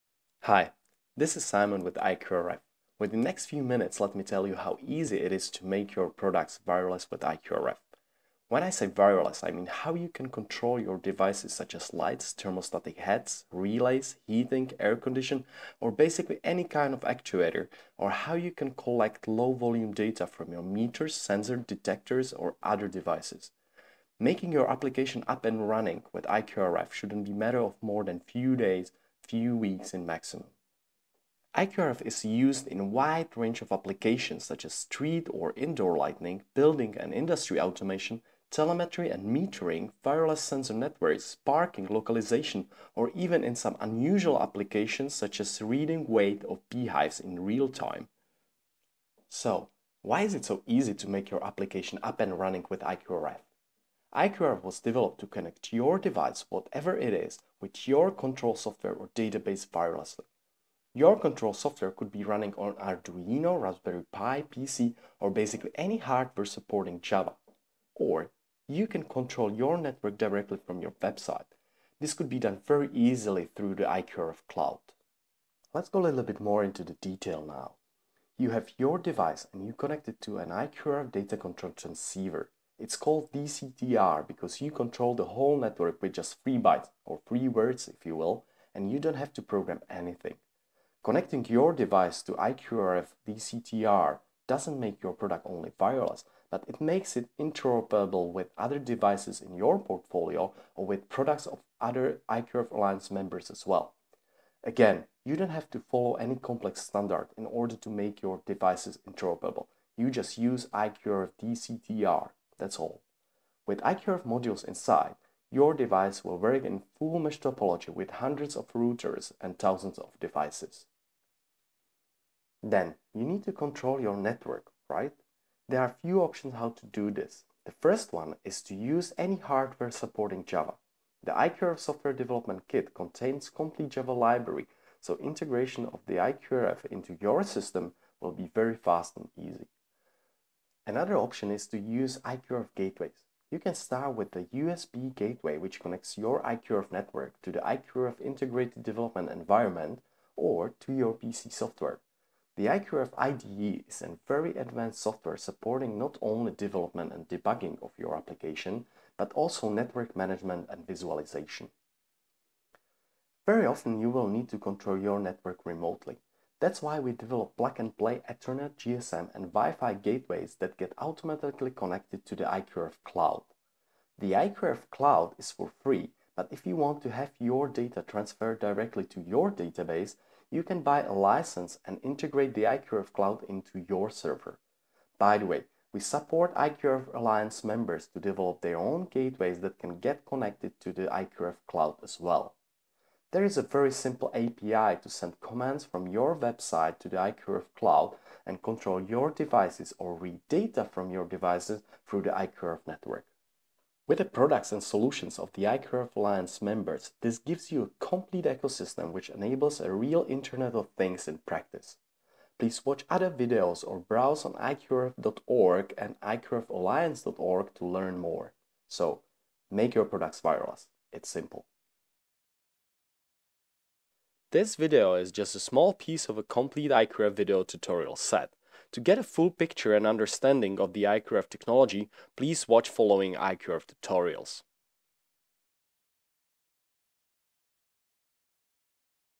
1. IQRF: To make your products wireless. Simply.
This is the first video of a complete IQRF Video Tutorial Set. It provides a general overview of the IQRF solution how you can make your products wireless and part of the Internet of Things Ecosystem.